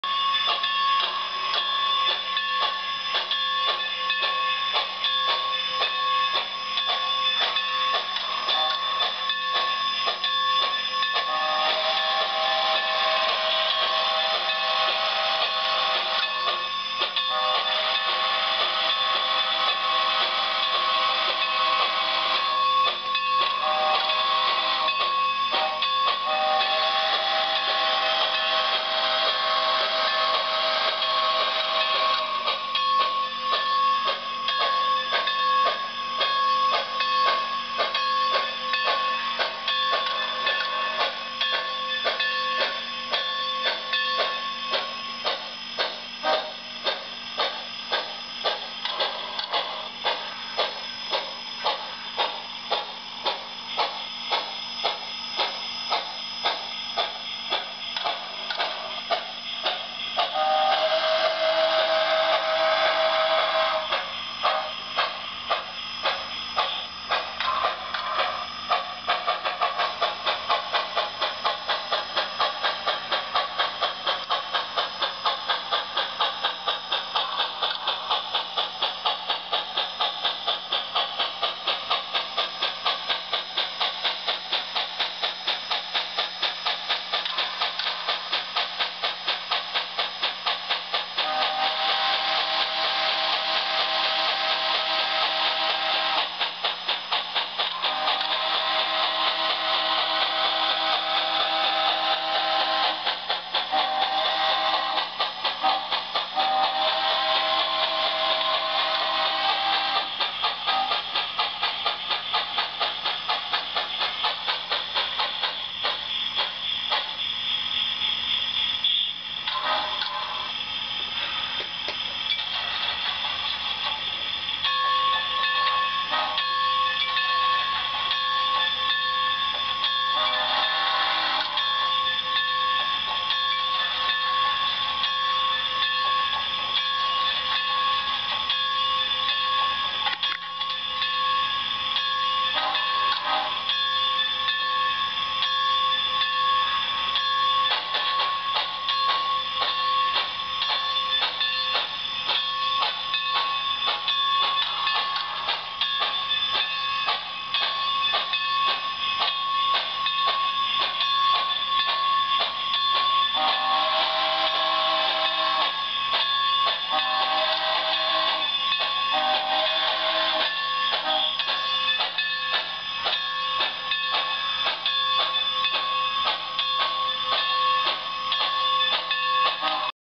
WM Ho 2-8-0 w/ 3 Chime Hancock
Its my Bachmann WM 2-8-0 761  with a 3 chime hancock, from soundtrexx with their heavy steam decoder. The specker is from a G-scale decoder  just fun.